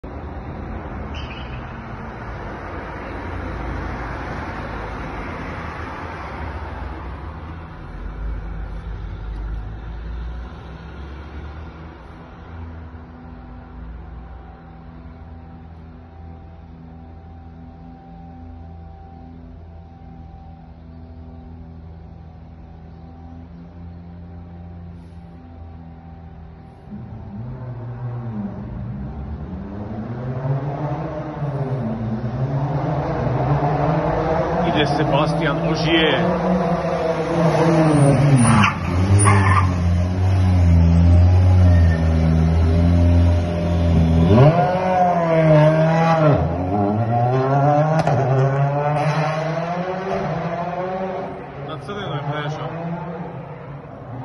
Sebastien Ogier goes through red light
Rally 2021
Sebastien Ogier goes through red light in Zagreb.

Right before this he was envolved in car crash. He took a victory in the end.